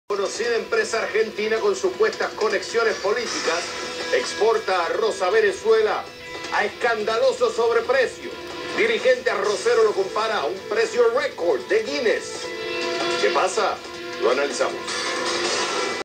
DEVIDO Y EMPRESA EXPORTADORA ASOCIADOS PARA CURRAR A LOS VENEZOLANOS
EL MINISTRO DE VIDO ETARIA EJERCIENDO SU PODER K ESTABLECIENDO UN "GANCHO" CON UNA DE LAS HIJAS DE CHAVEZ DE VENEZUELA Y LE VENDE A ESTE PAIS A PRECIOS SOBREFACTURADOS EL ARROZ Y EL MAIZ A TRAVES DE UNA EMPRESA EXPORTADORA QUE NO SE DEDICABA A LA EXPORTACION..PARA MAS INFORMACION VEA EL VIDEO  SE ESTARIA USANDO EL NOMBRE O LA MEMORIA DE LOS JUERTOS COMO CHAVEZ Y NESTOR PARA SEGUI9R CURRNDO GUITA AL PUEBLO , EN ESTE CASO CIRRANDO EN LOS PRECIOS DEL ARROZ Y EL MAIZ PARA VENEZUELA QUE ESTA SUFRIENDO CAOS SOCIAL, DESOCUPACION Y POBREZA..VEA ESTE VIDEO PARA MAS INFORMACION